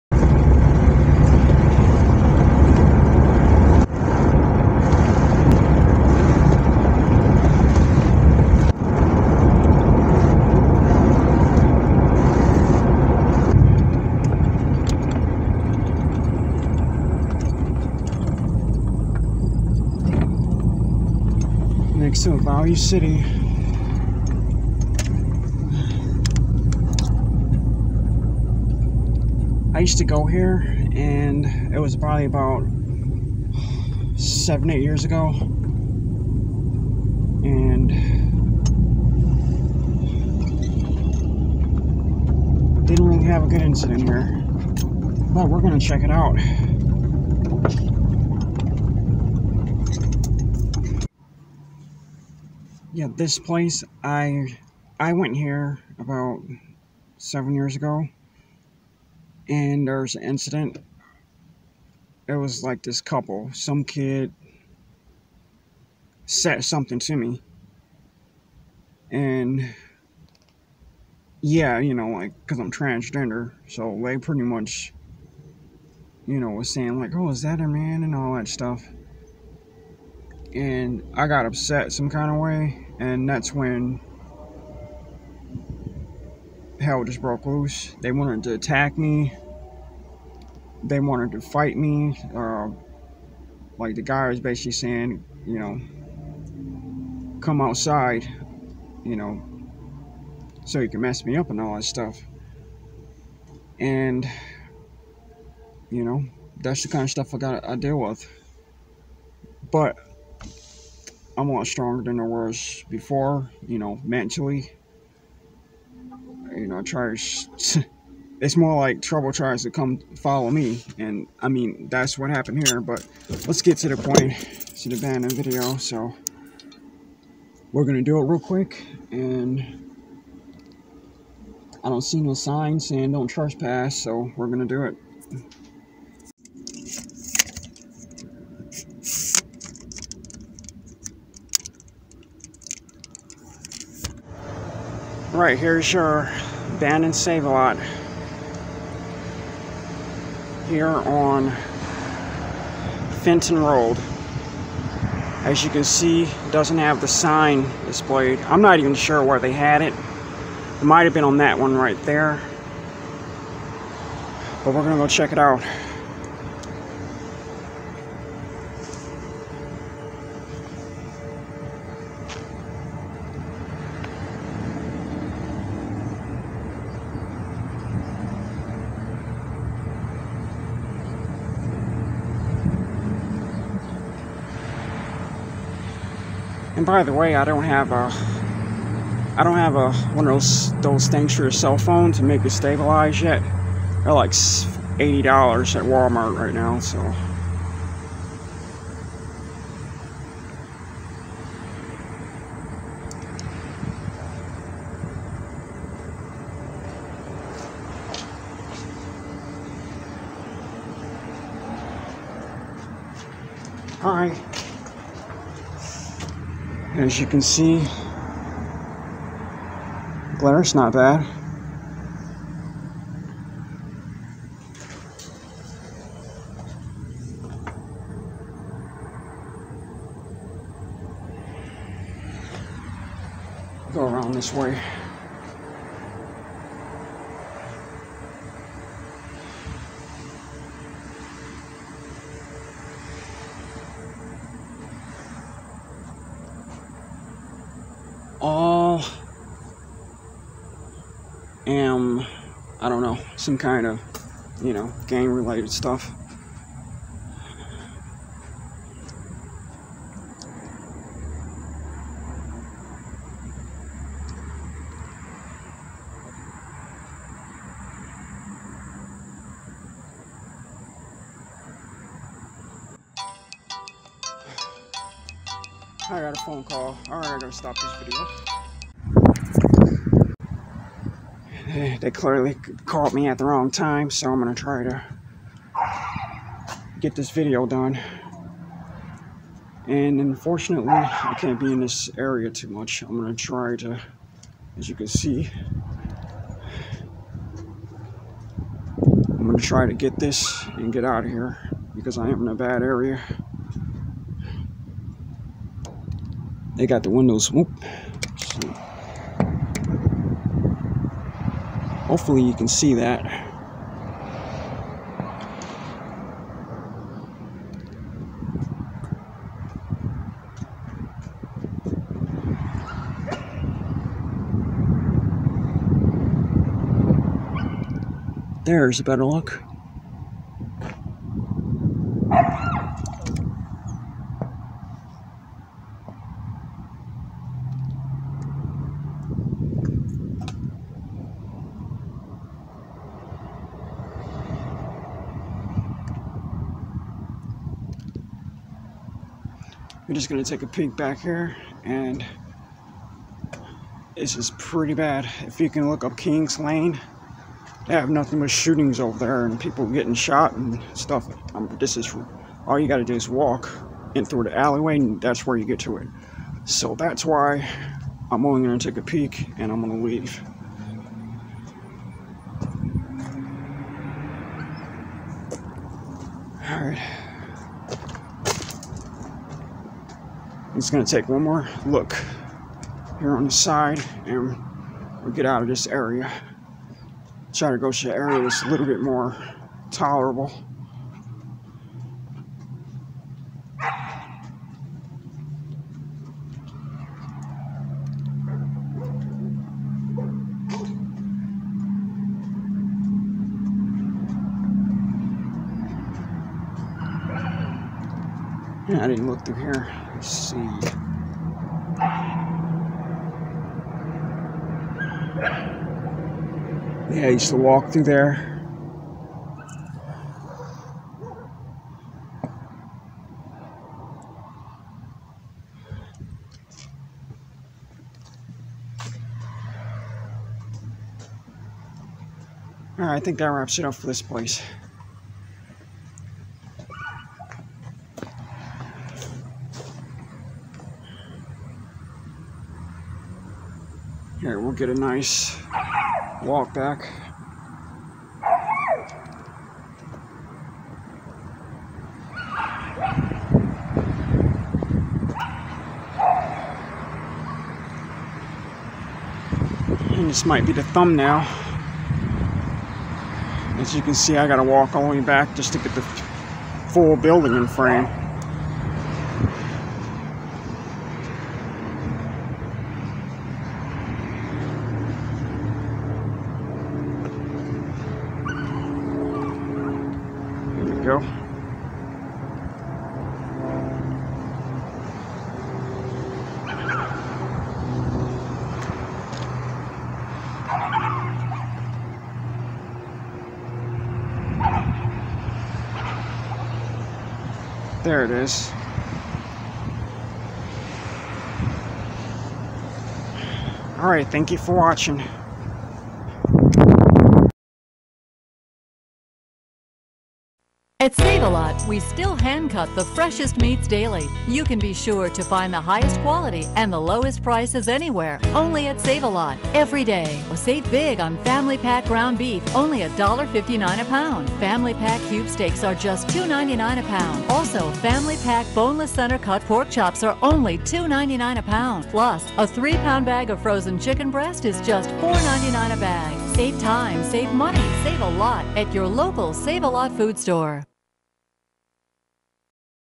Save A Lot Grocery Store / Abandoned - Flint MI
I use to go to this save a lot with my friend at the time but know it sits abandoned and tagged up. Located at South Flint Plz, 5125 Fenton Rd, Flint MI. Had some great memories there and bad ones to but the food prices were always pretty good. Not sure when it closed but had to be sometime after 2016.